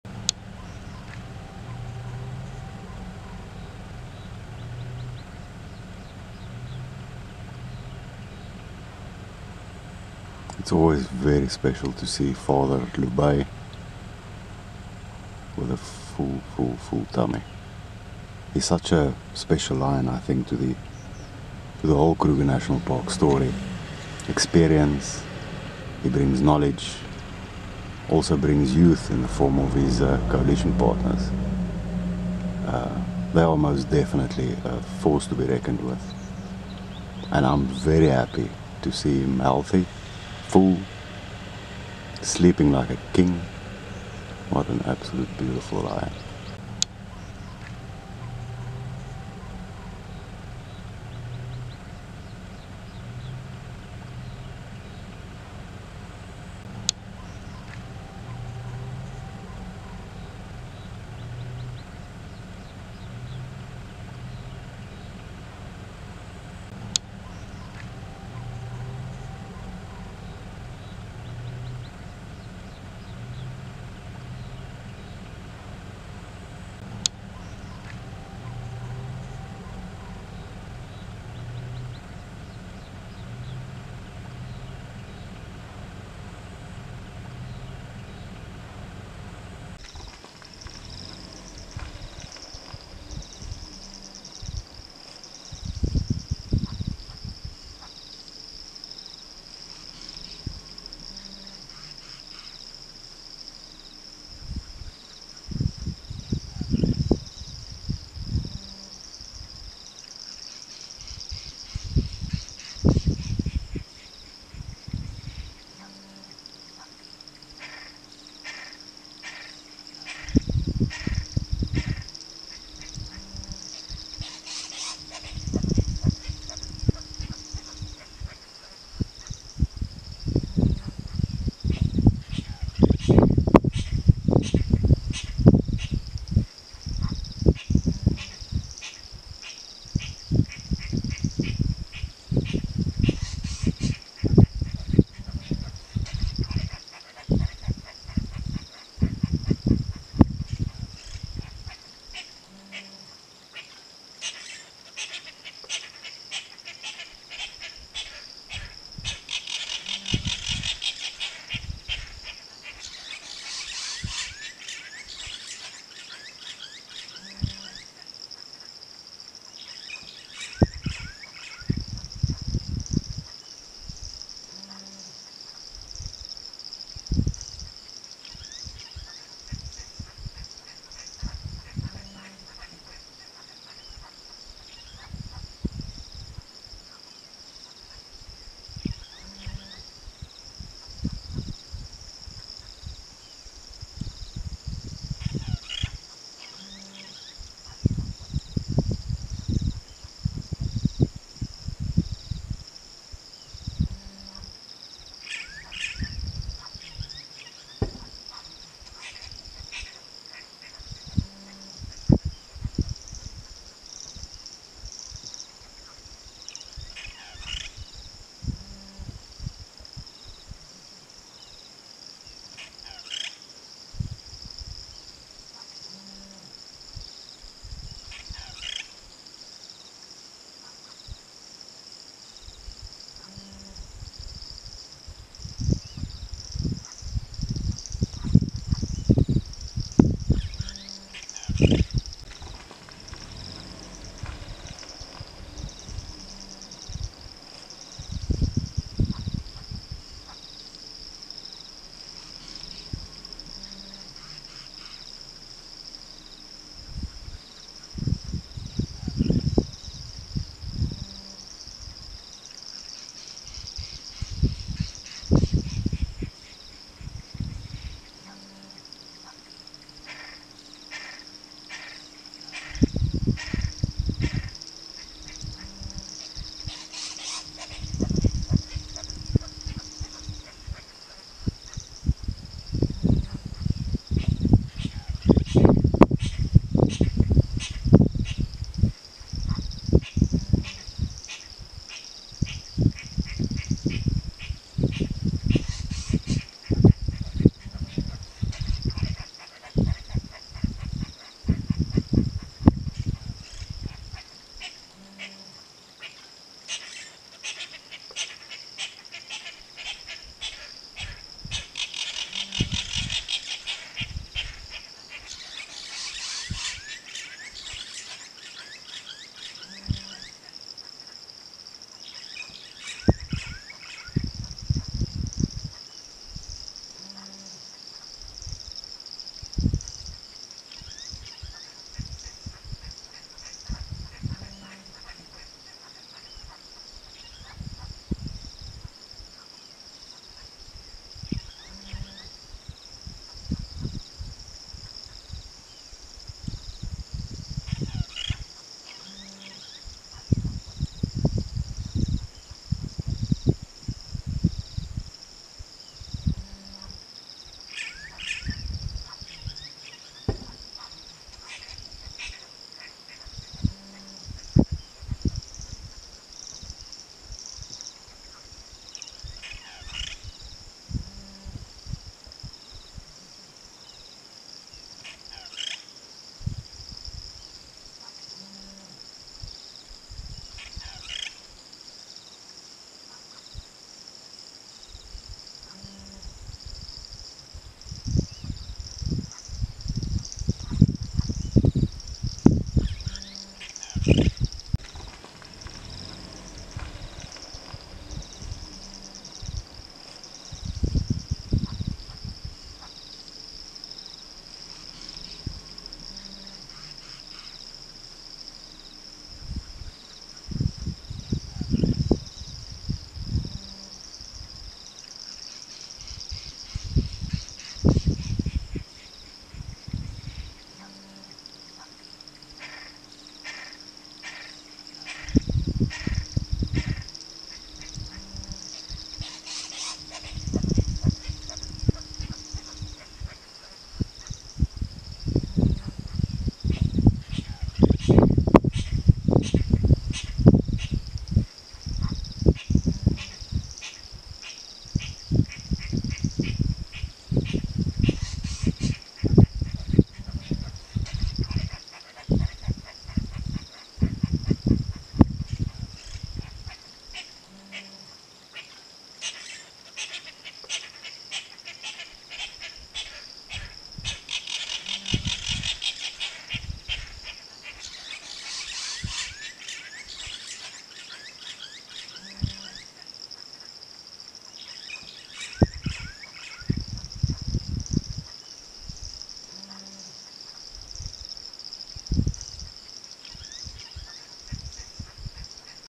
LAST MOMENT OF A MALE LIONS LIFE! Lubeylubey Lion Has His Last Moments! Kruger National Park!lion
Today, I bring a heart shattering video, one I don't really know how to title or describe really. I had a discussion with my older boy just the other day, and I told him that I dread the day to find one of my favorite Male Lions like this, but it was inevitable. Father LubeyLubey, the Old Male Lion, The Father Lion of the Kruger National Park is in a horrible condition, after a fatal fight with other Male Lions(possibly 3 to 4 lions, looking at the bite marks). One can clearly see two badly infected wounds on his hindquarters(bite marks), and he can barely use his back legs at all, as the spine is also injured badly. Reports from a ranger this morning: He told me that he saw two males with heavy scratch marks on their bodies the previous night, not far from where Lubye was seen today - Males unknown thus far. What a sad sad day, that I found this beautiful boy in this way, after his nearly 4 month disappearance. He stood up for me one last time after 3 hours just laying down, as if to say to me and show me, "Remember me for what I was". My heart is broken, for today, the Kruger National Park has lost a legend Male Lion(not yet dead, but unlikely that he will live the full day today). I'm absolutely heartbroken, as I've followed him for a long time. I've had extremely great times with him, and will remember them vividly, each passing day. May he go peacefully, without any further injuries. Rest easy, my father lion.

P.S - I really don't have the energy or power to deal with uneducated comments today, about how I should have helped him ect. This is a wild Male Lion. No one is allowed to intervene with nature in the Kruger National Park, it has never been allowed, no matter how much you hate it. This is Lion coalition/territory fights, it happens. Many warrior Male Lions have fallen the same way. He will go as intended by nature, as sad as that is, to me specially.